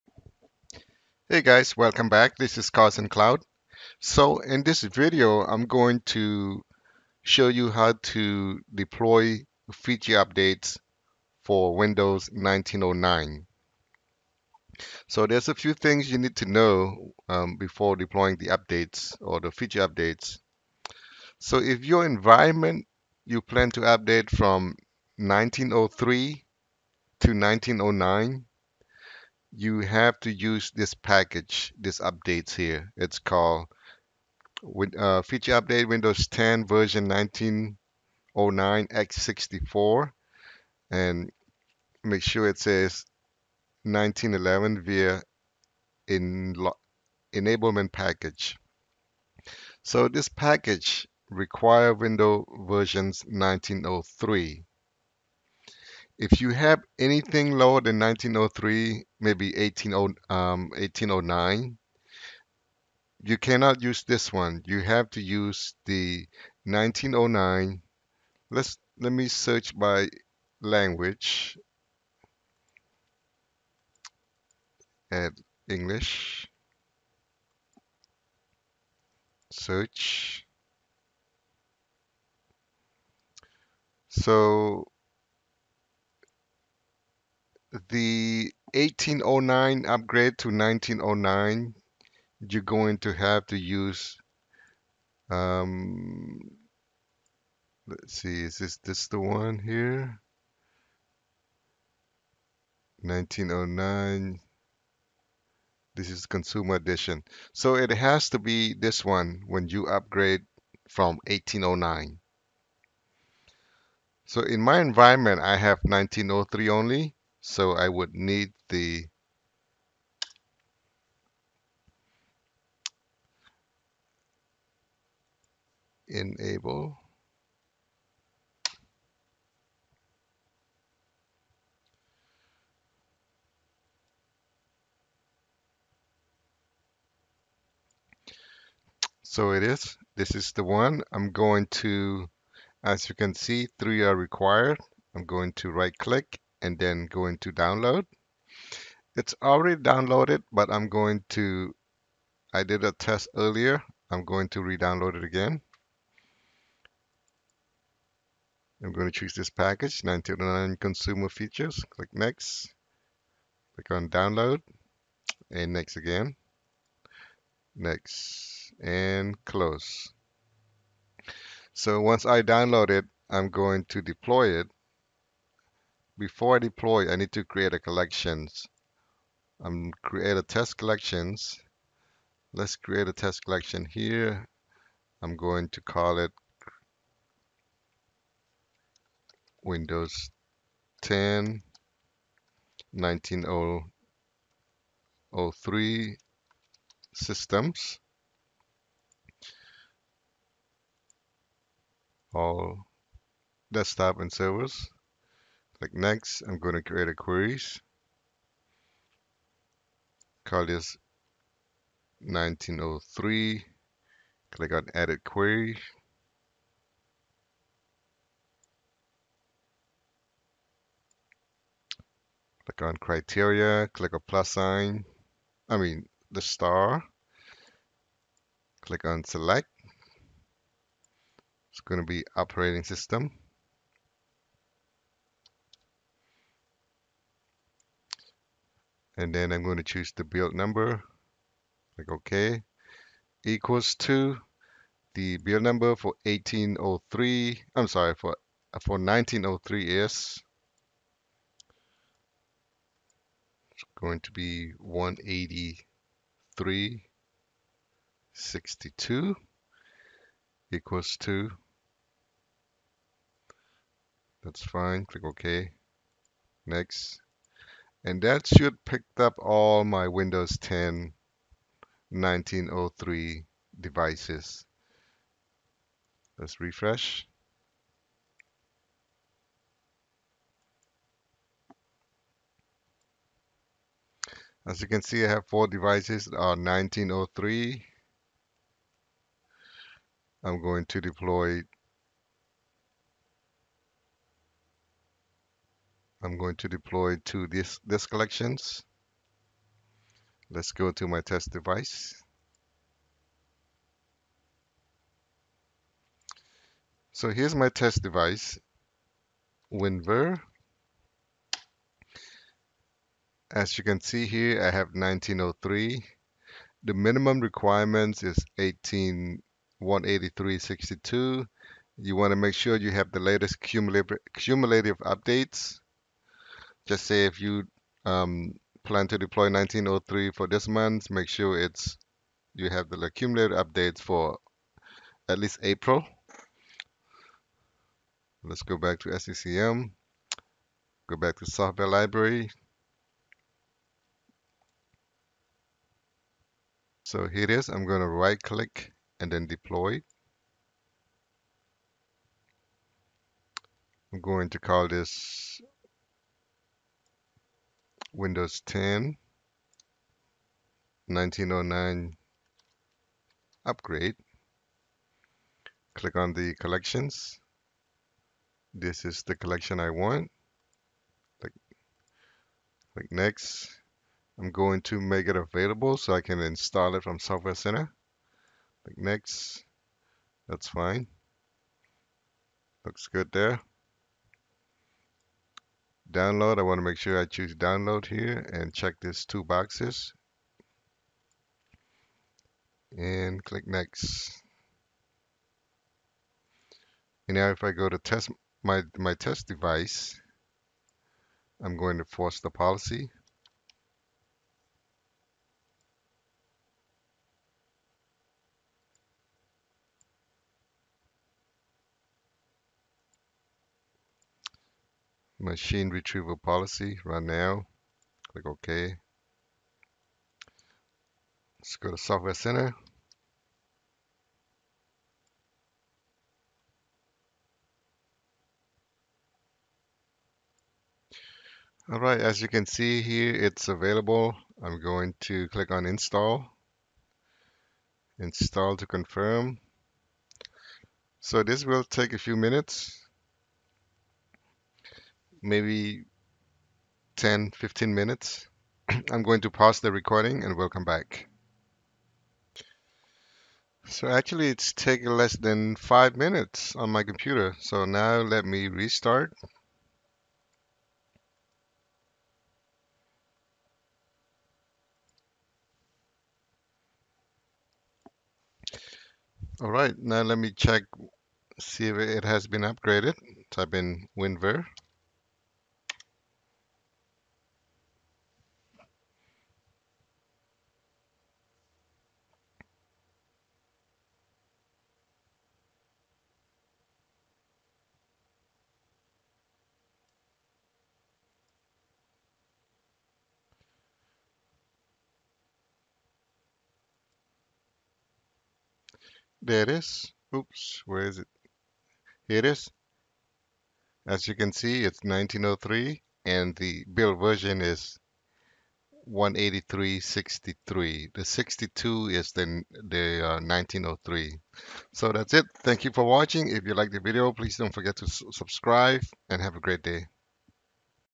Deploy Windows 1909 Feature upgrade in SCCM
How to upgrade to windows 10 1909 in SCCM using Windows update.